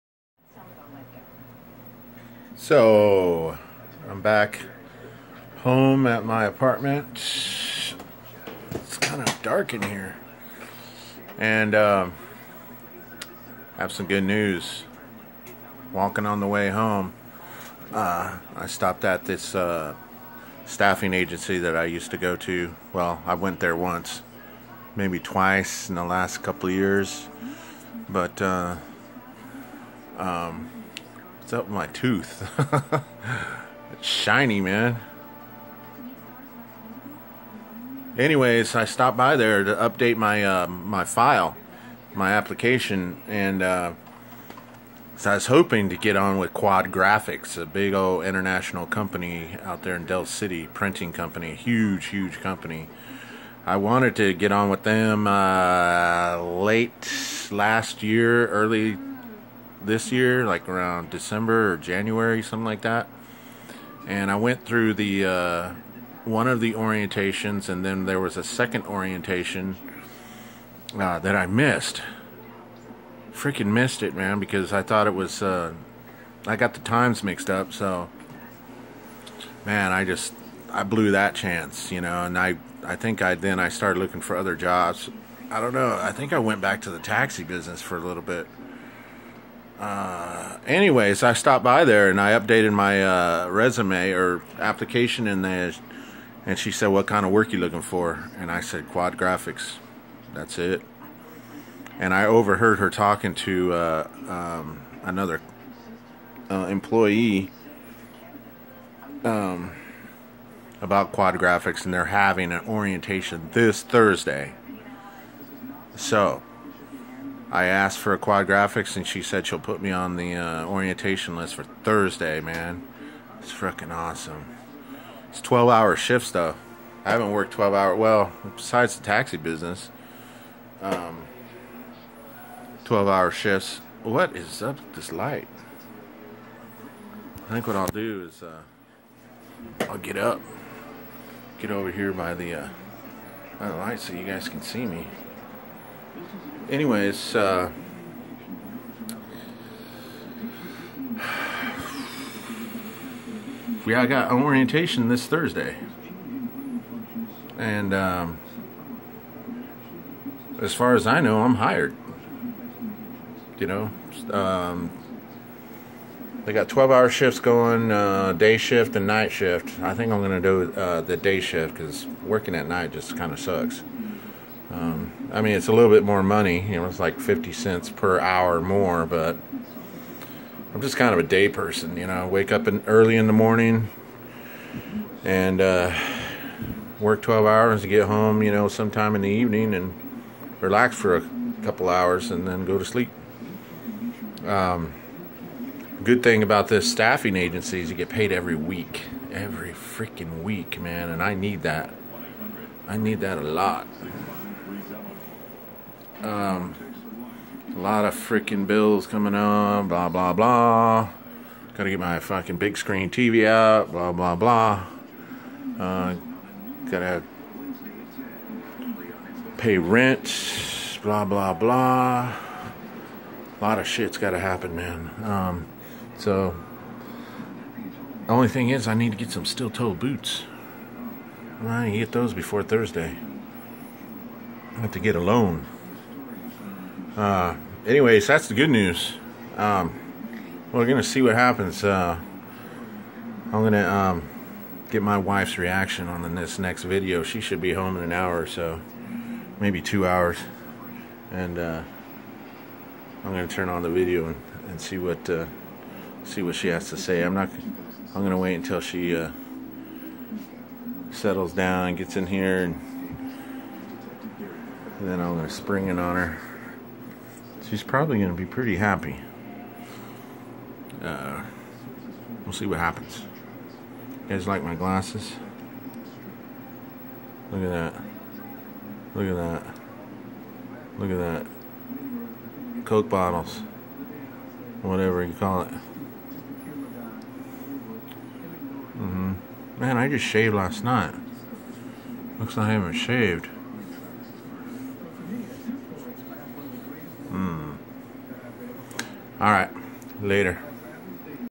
Job search in Oklahoma City. Day One.
Whoa. I think I just landed a job at Quad Graphics.